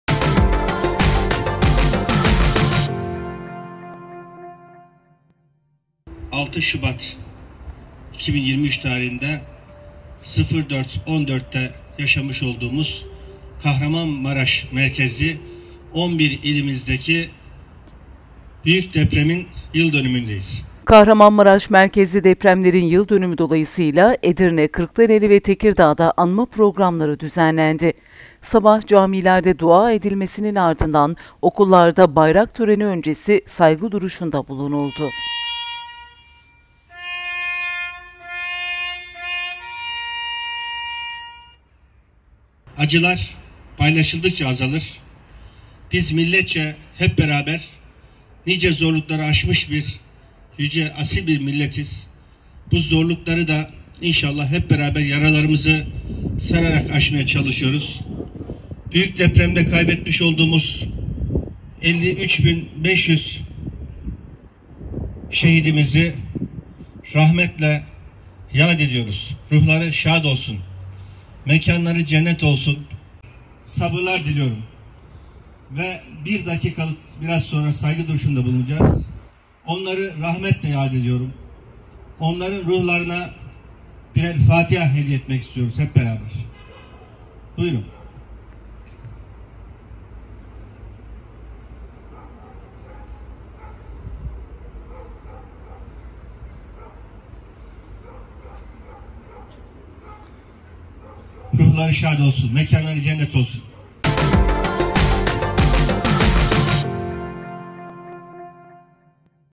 Depremde hayatını kaybedenler anıldı
Kahramanmaraş merkezli depremlerin yıl dönümü dolayısıyla Edirne, Kırklareli ve Tekirdağ'da anma programları düzenlendi. Sabah camilerde dua edilmesinin ardından, okullarda bayrak töreni öncesi saygı duruşunda bulunuldu.